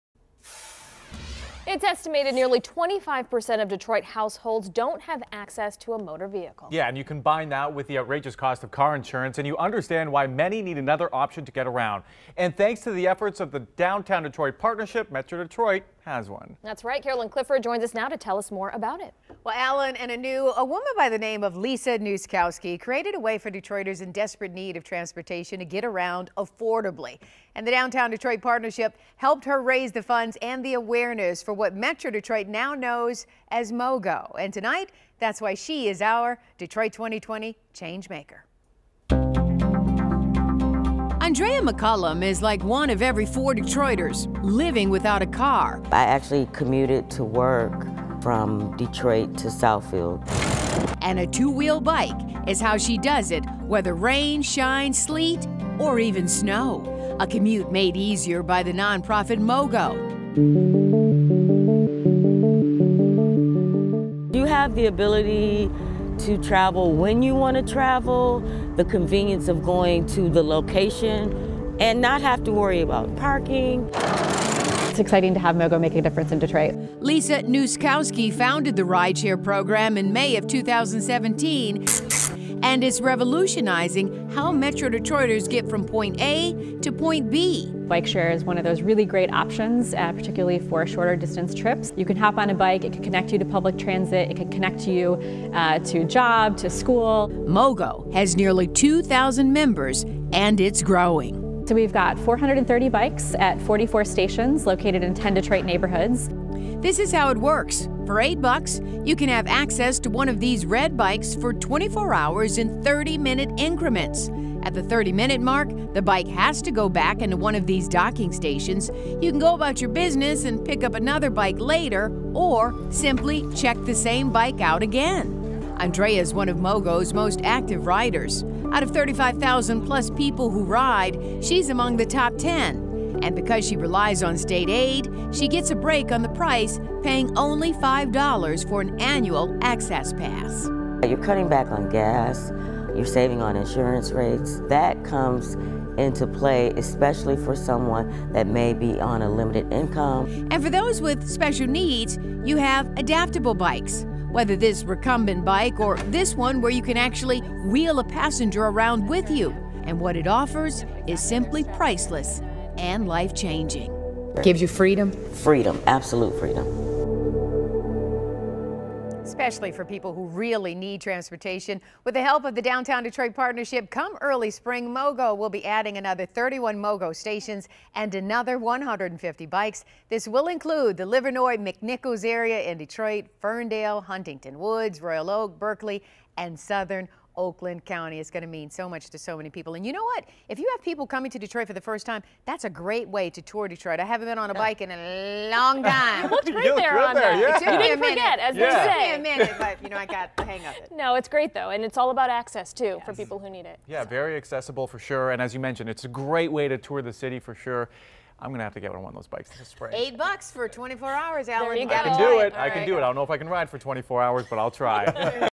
Detroit 2020 Creator of MOGO is a Detroit 2020 Changemaker
WXYZ Detroit Channel 7
MoGo founder and Executive Director, Lisa Nuszkowski, was recently selected as WXYZ Channel 7’s 2020 Changemaker of the Week! We are so proud of the work she’s done.